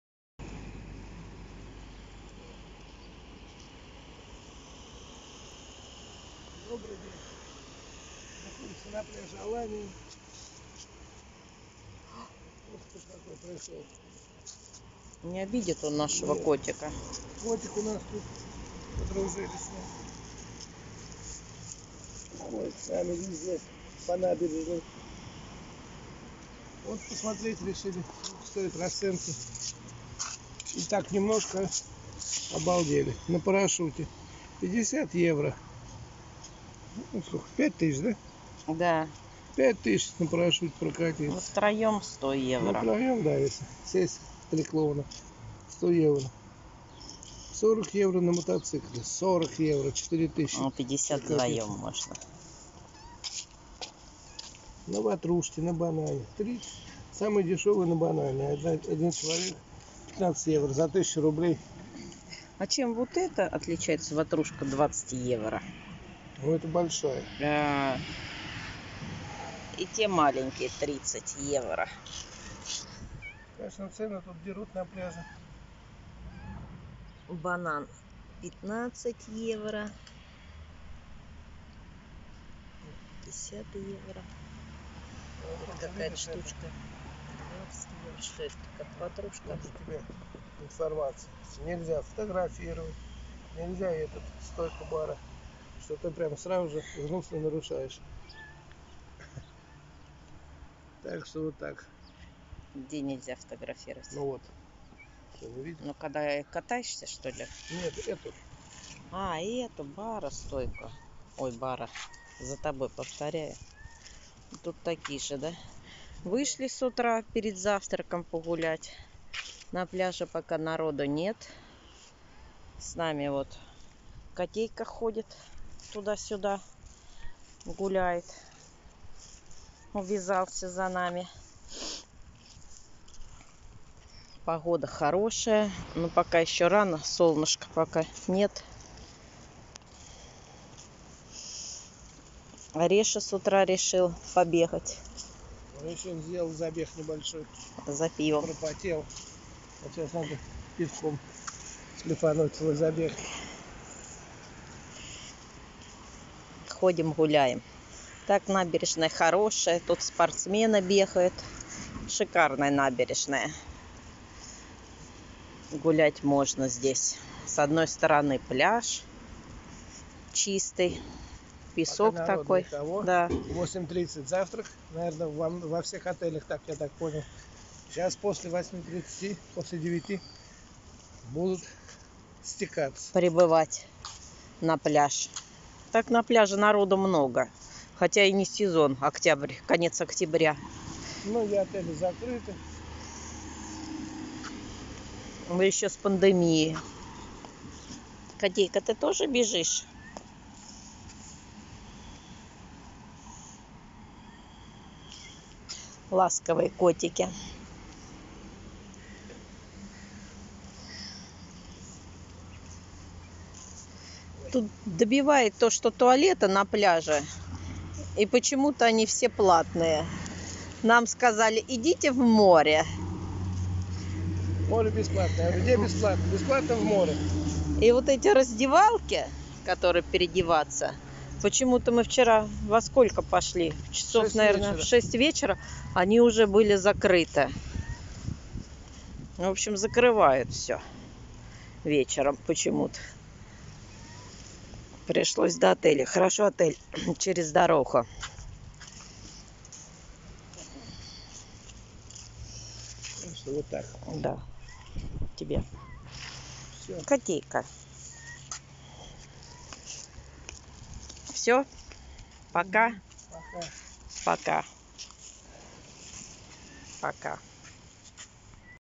Турция,Анталья, Аланья,Kleоpatra Sahara Hotel, прогулка по набережной!
Приехали в Турцию!